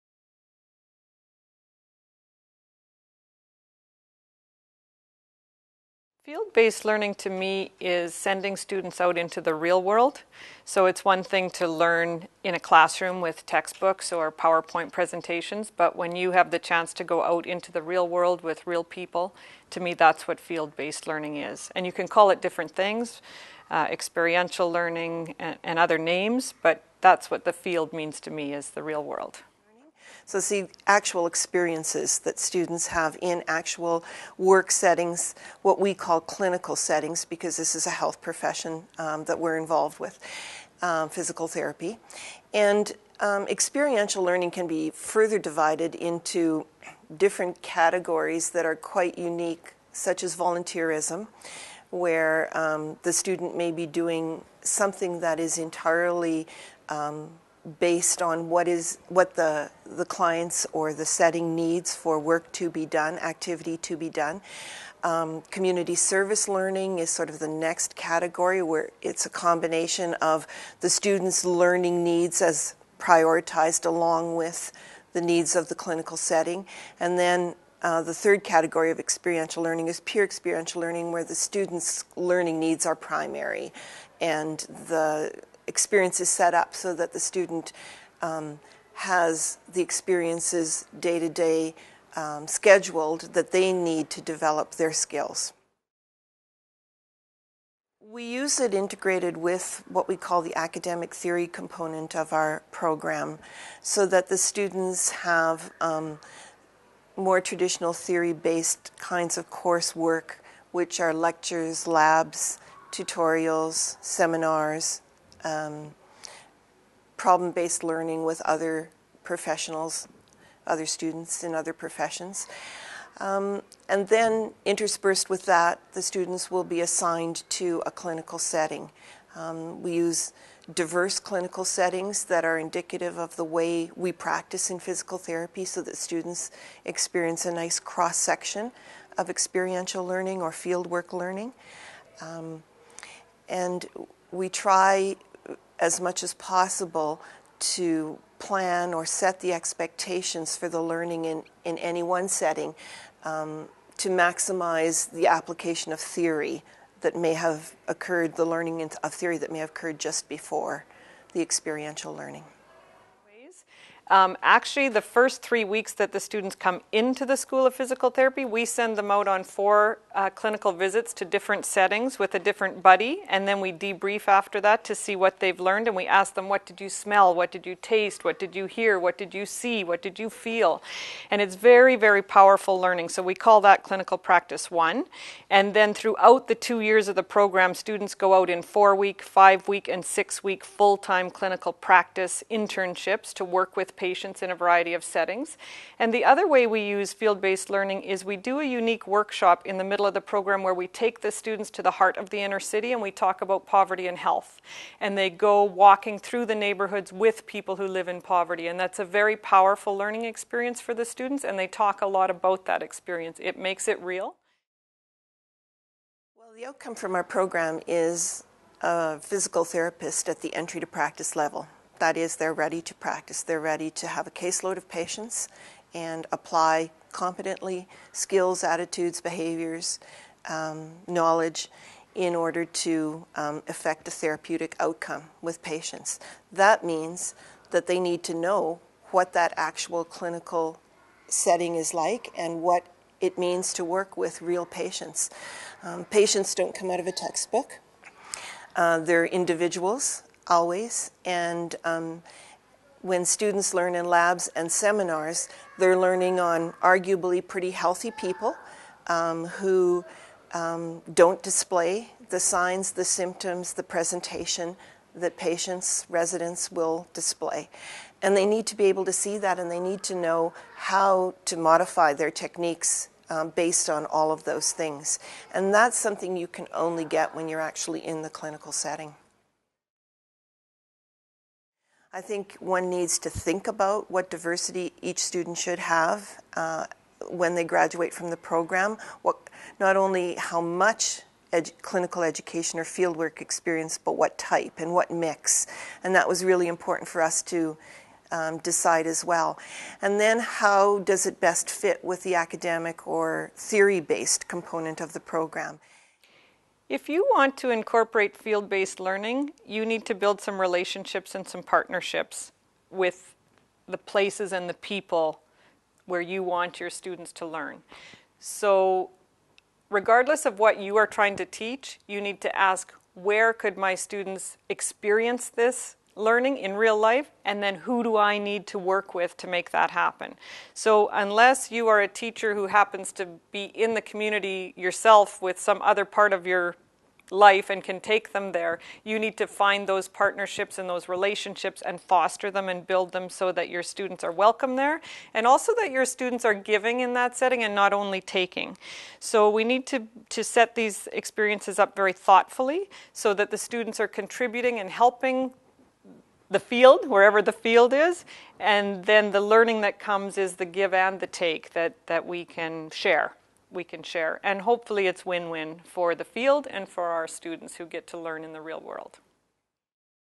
Field Based Learning in the School of Physical Therapy
Interview with Peggy Proctor and Arlis McQuarrie of the  University of Saskatchewan as they share their experiences with incorporating Field Based Learning in the College of Physical Therapy.

This video has been created by The Gwenna Moss Centre for Teaching Effectiveness at the University of Saskatchewan.